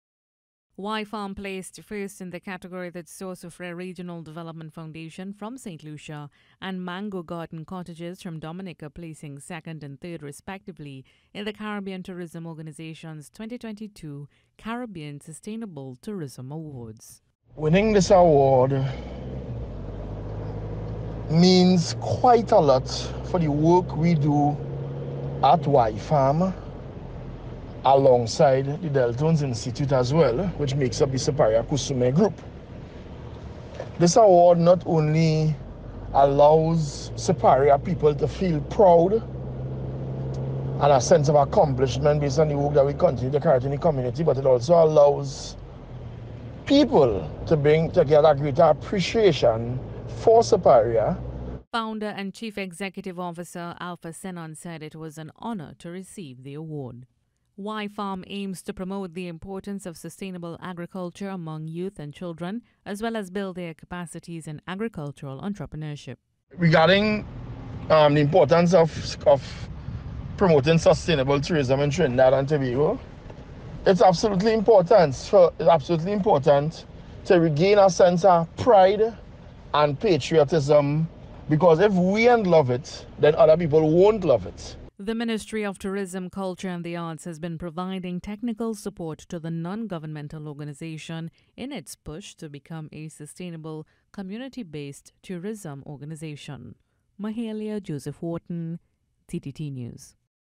WHYFARM Wins 2022 CTO Award
The Ministry of Tourism, Culture and the Arts has extended congratulations to We Help Youth FARM, or WHYFARM of Siparia on winning the prestigious Community-Based Tourism Award category in the Caribbean Tourism Organization’s (CTO) 2022 Caribbean Sustainable Tourism Awards.

Founder & CEO Alpha Sennon said it is important to promote and preserve or landscape and environment – so that visitors can also enjoy it.